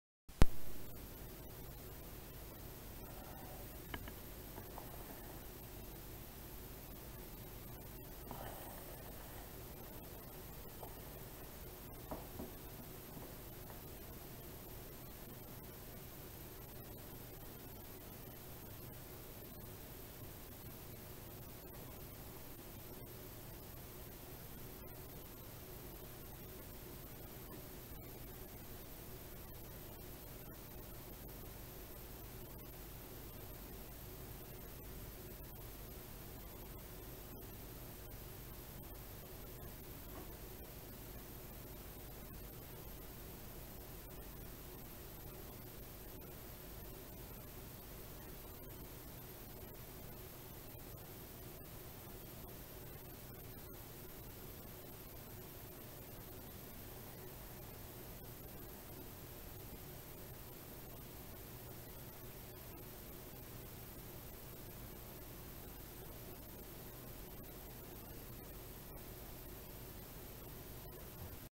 Pend 903mm
A pendulum of length 0.903m
There are at least 30 oscillations recorded
Use the join in the surface behind the pendulum as a fiducial mark.